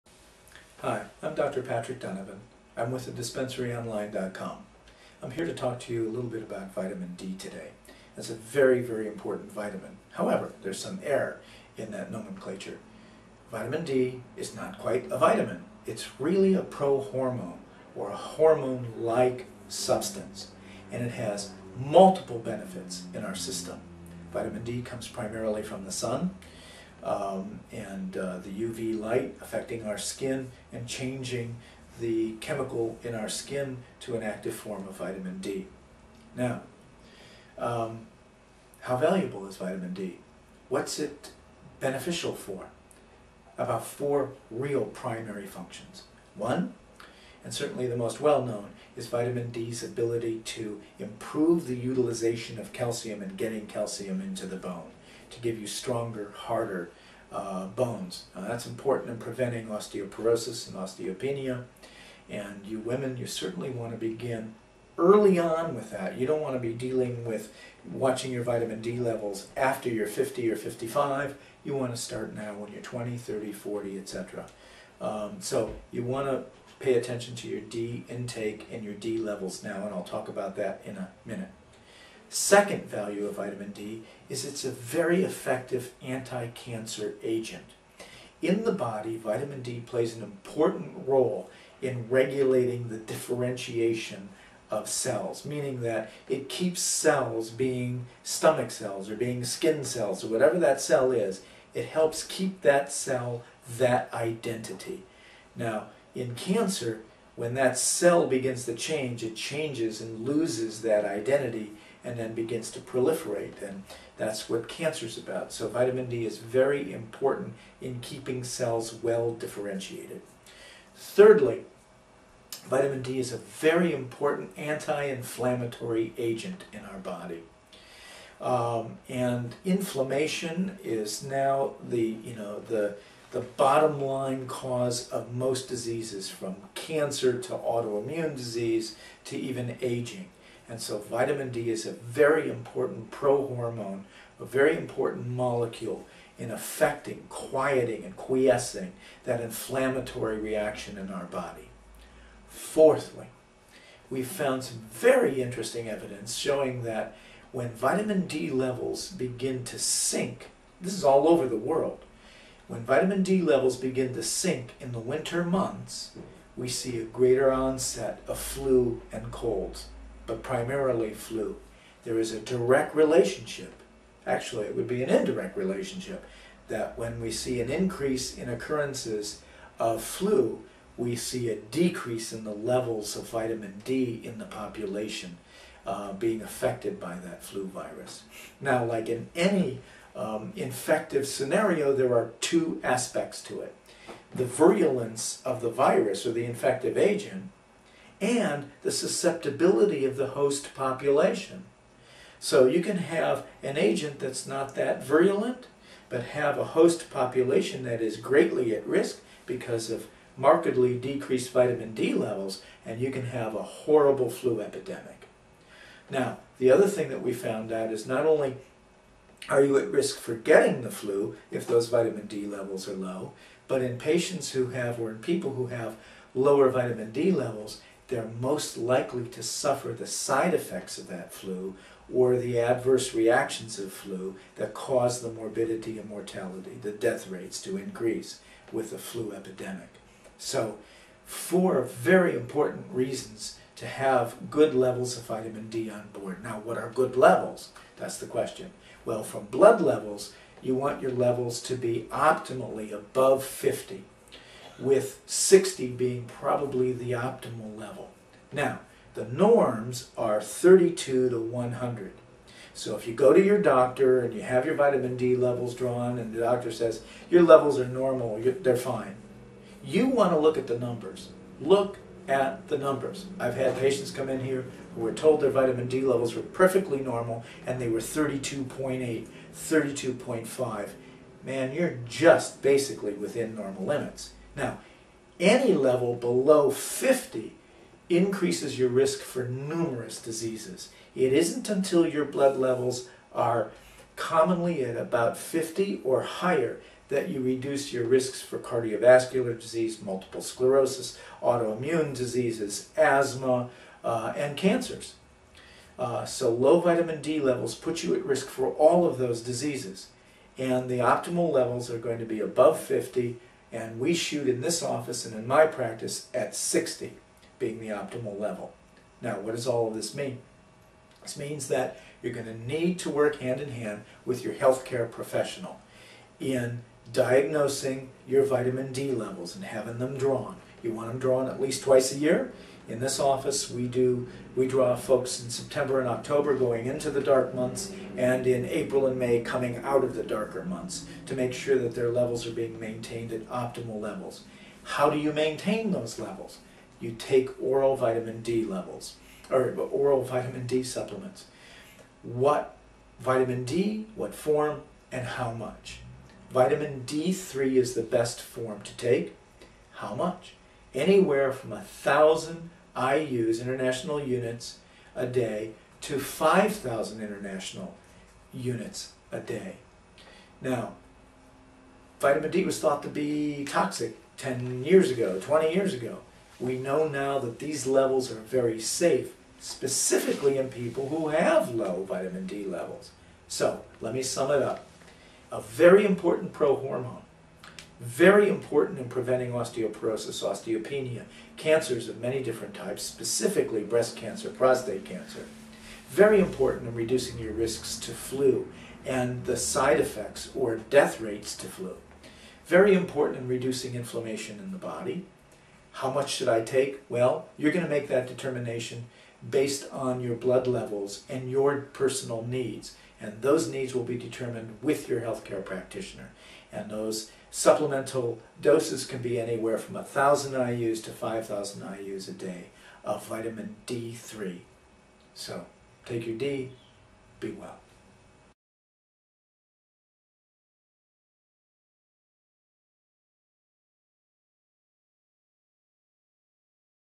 Vitamin D and Why You Should Take It
Dr. Patrick Donovan of talks about the many benefits of Vitamin D.